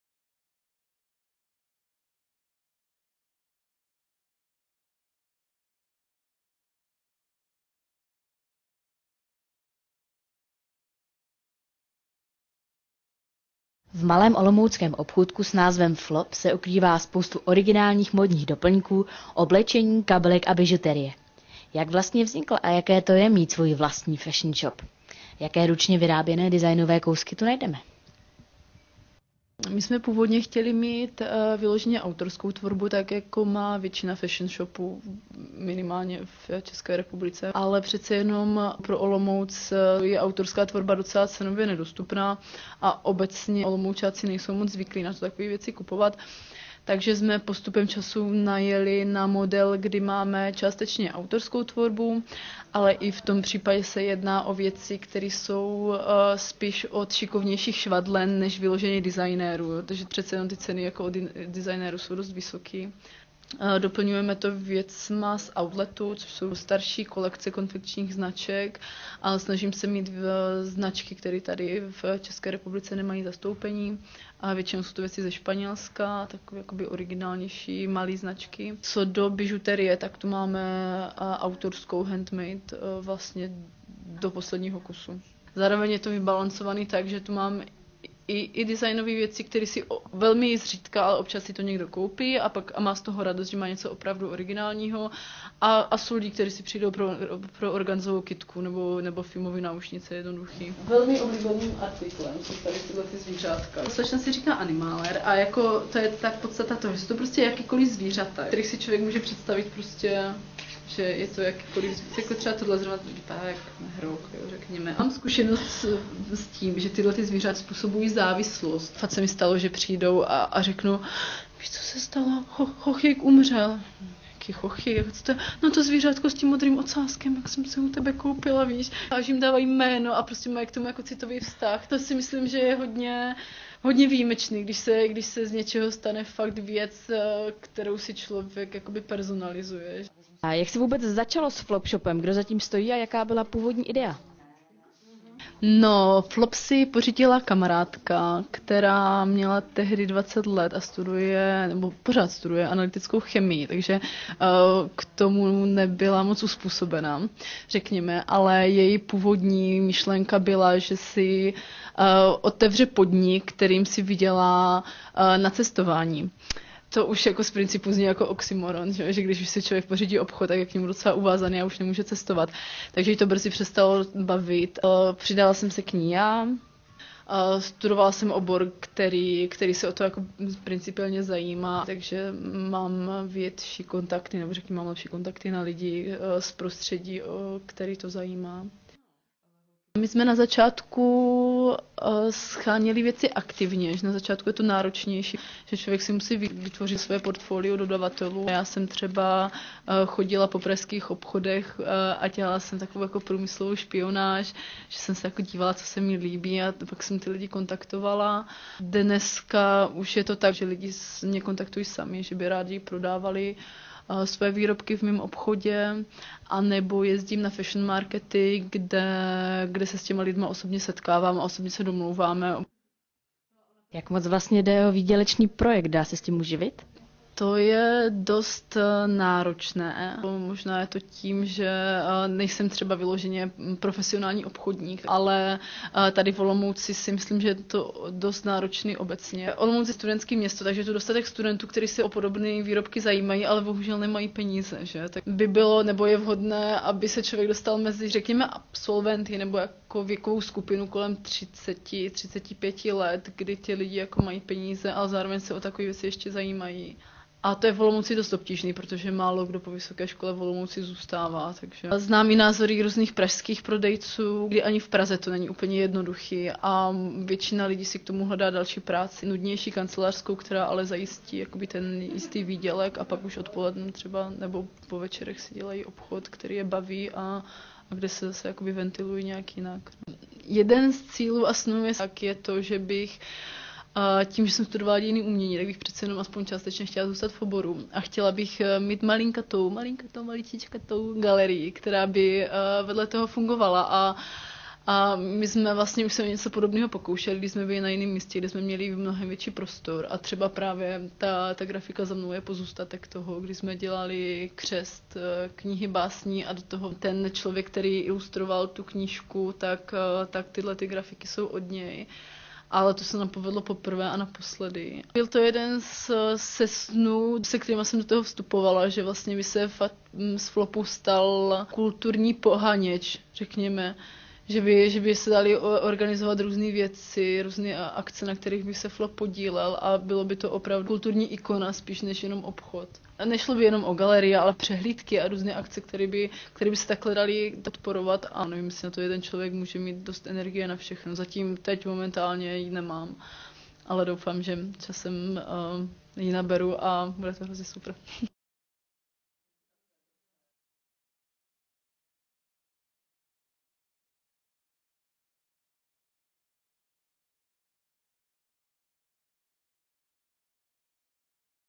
Flop - Kousek umění
V malém obchůdku Flop v Olomouci se ukrývá spousta originálních módních doplňků, oblečení, kabelek a bižuterie. Jak vlastně vznikl a jaké je to mít svůj vlastní fashion shop? Co lze najít?

srpen 2012